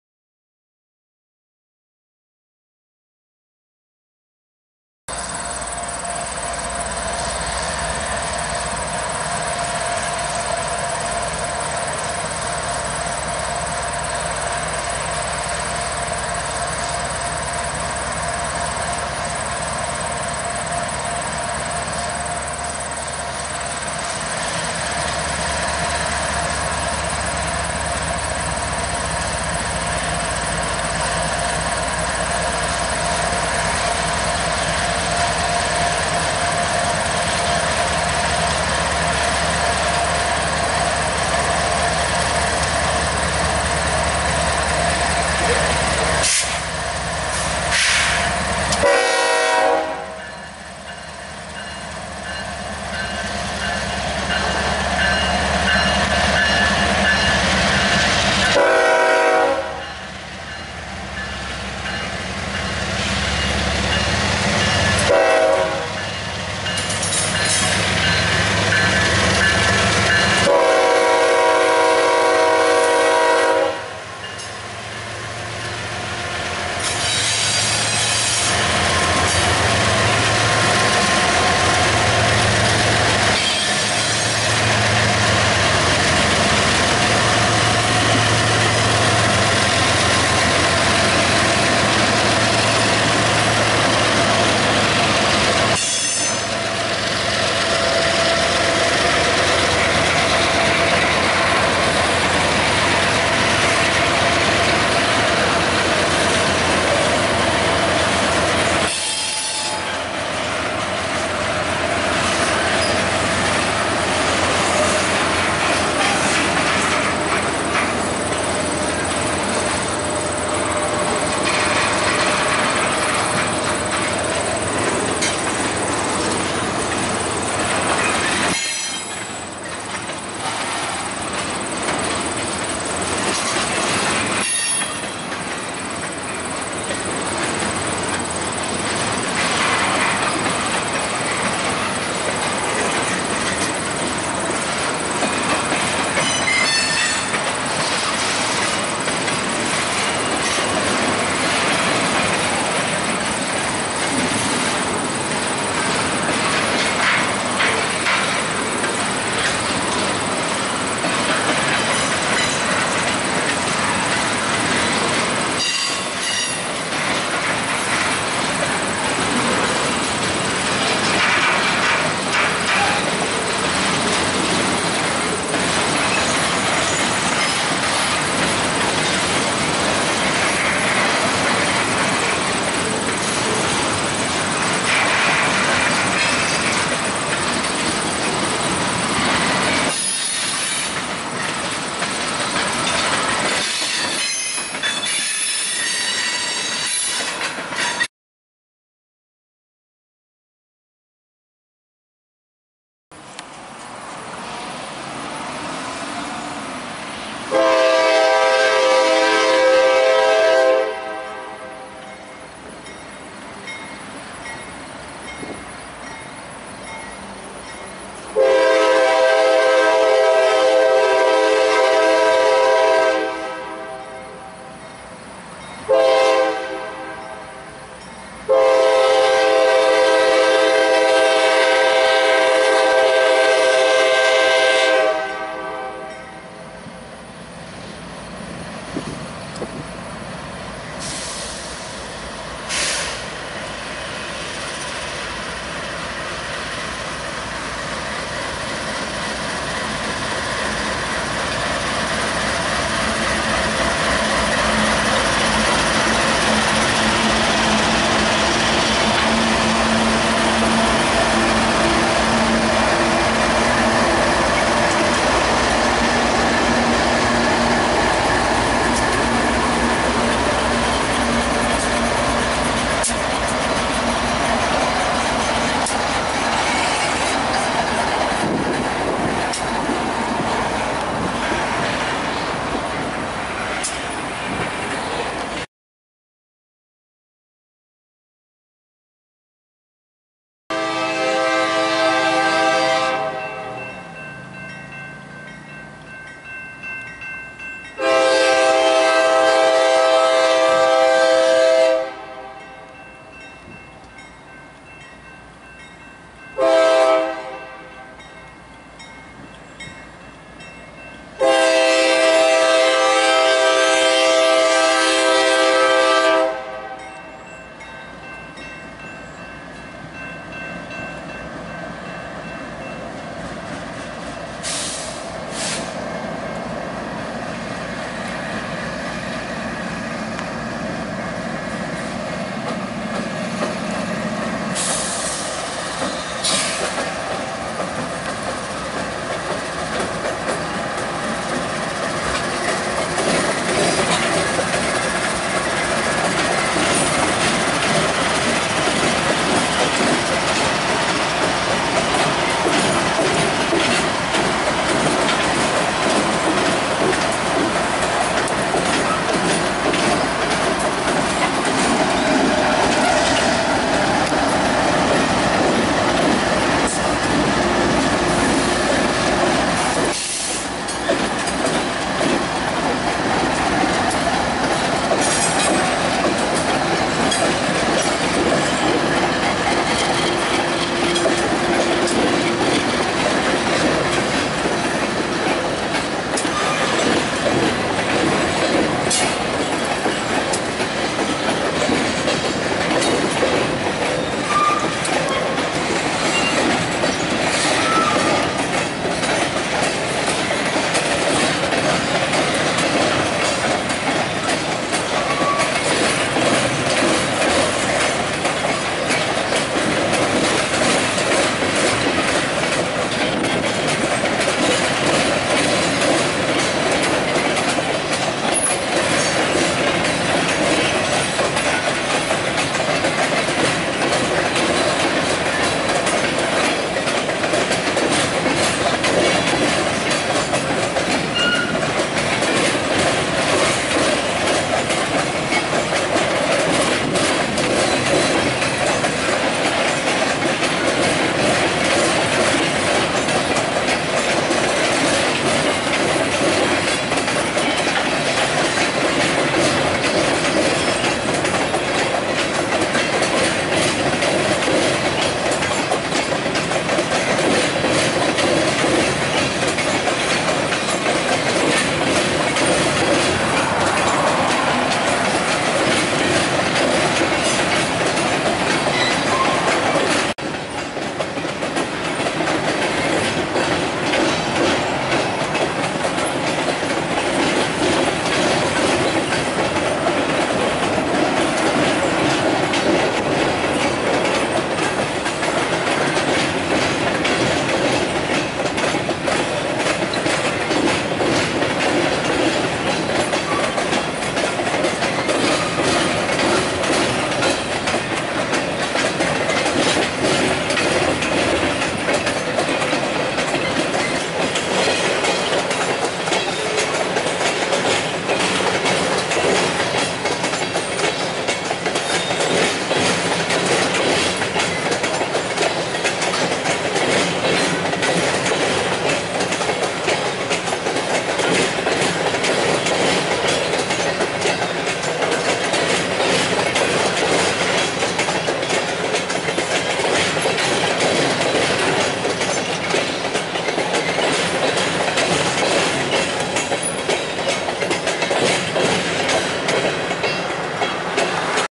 IAIS CBBI meets PEDM at Moline siding September 3, 2019
IAIS CBBI (Council Bluffs to Blue Island) meets PEDM (Peoria to Des Moines) at Moline siding. After crews swap trains the CBBI crew realigns the siding switch to continue onto Silvis. PEDM is seen near the TaxSlayer center in Moline and then again at Rock Island yard. PEDM is a grain train off the NS at Peoria.